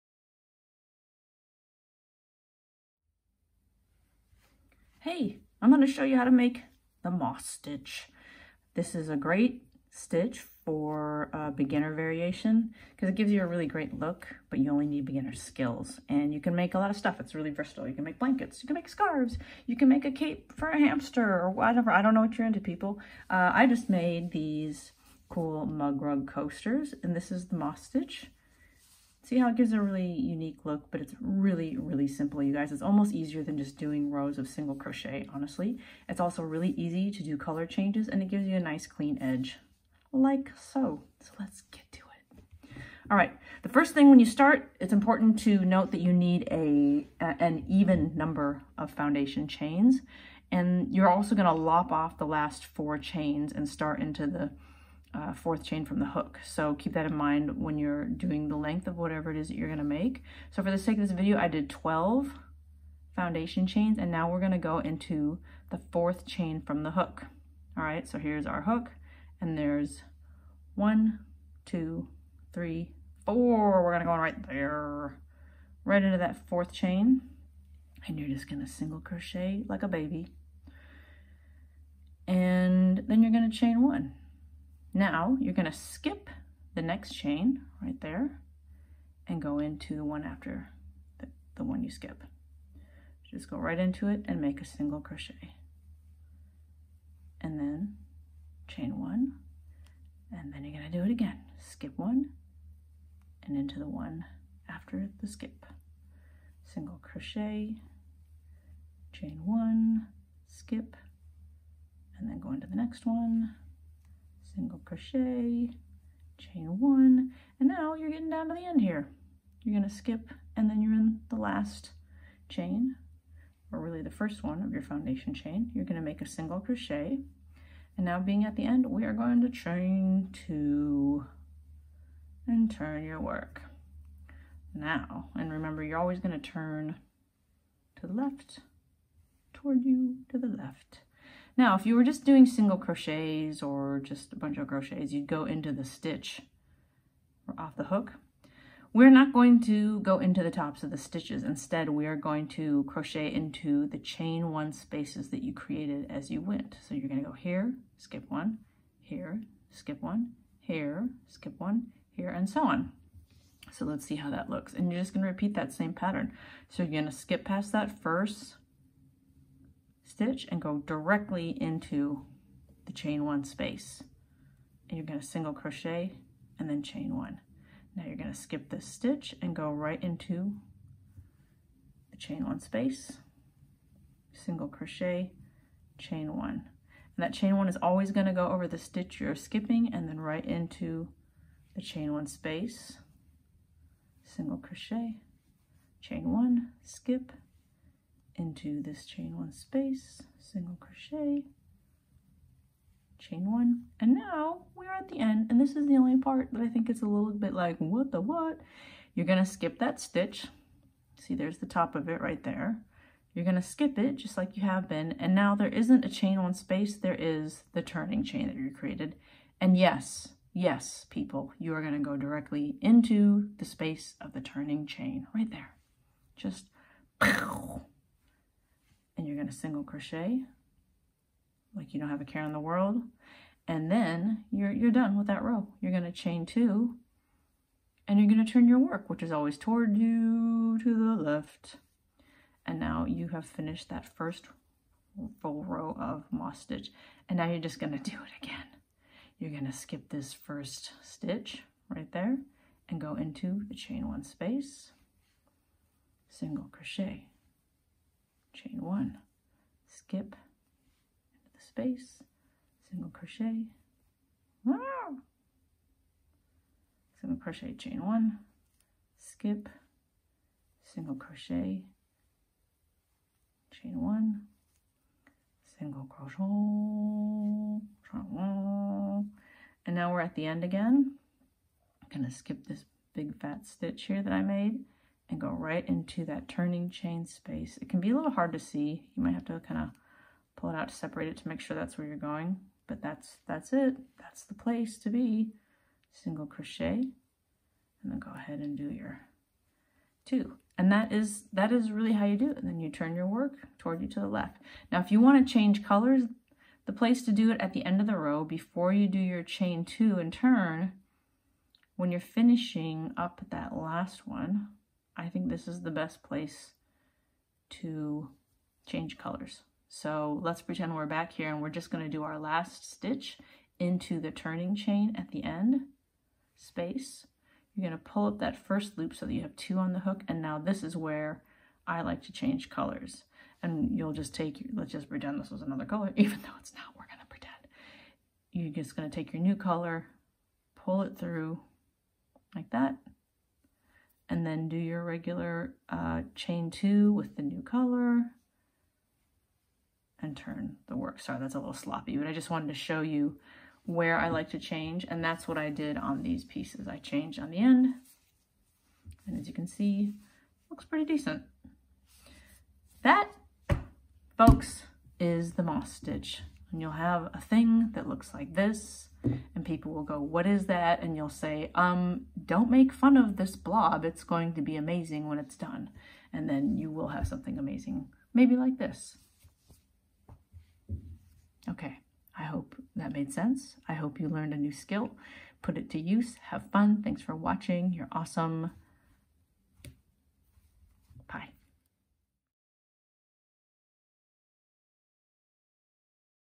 How to Crochet the Moss Stitch - Tutorial
The moss stitch is a great one to add to your beginner and intermediate repertoire. You can use it for lots of different projects and it’s soooo easy!

0:00 Intro
0:42 Chain
1:09 Row 1
2:00 End of Row 1
2:20 Row 2
3:48 End of Row 2
5:08 Where to place color change

I hope this tutorial was helpful for you :)

These are the crochet hooks I use!
Comfort Grip Hook Set :